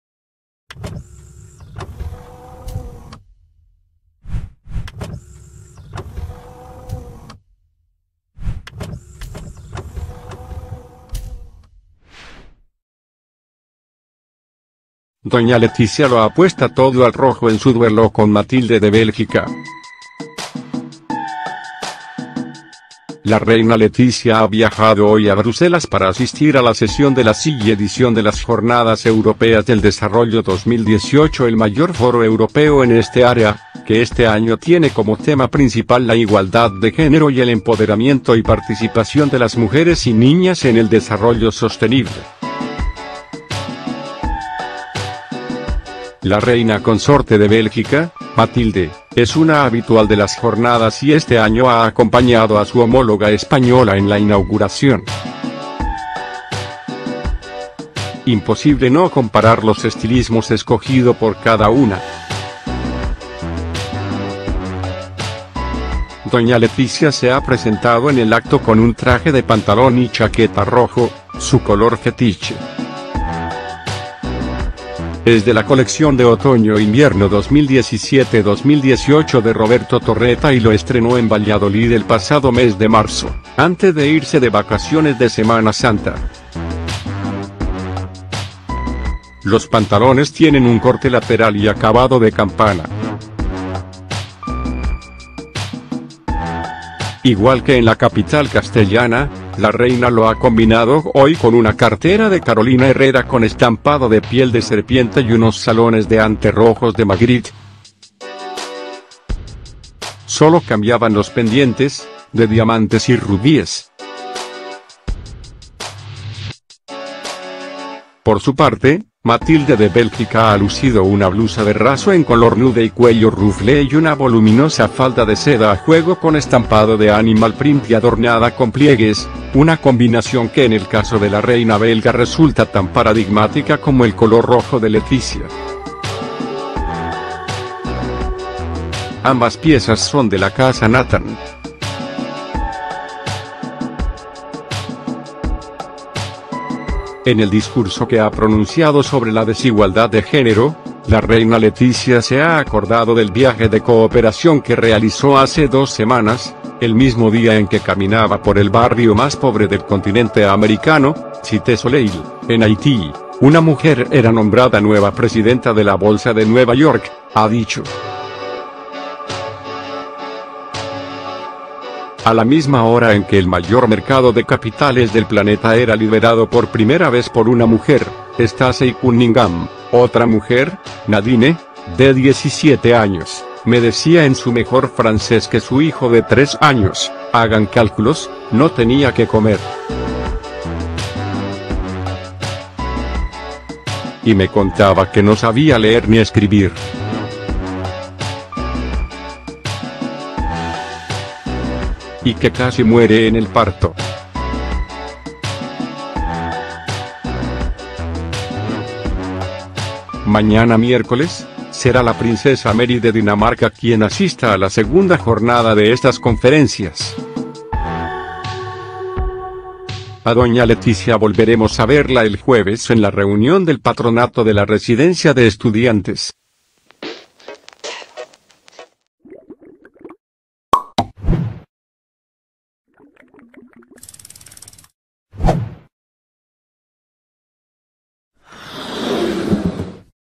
Noticias de hoy Doña Letizia lo apuesta todo al rojo en su 'duelo' con Matilde de Bélgica
La Voz 191:Doña Letizia lo apuesta todo al rojo en su 'duelo' con Matilde de Bélgica
Las dos reinas han asistido en Bruselas a la inauguración de unas jornadas sobre desarrollo sostenible.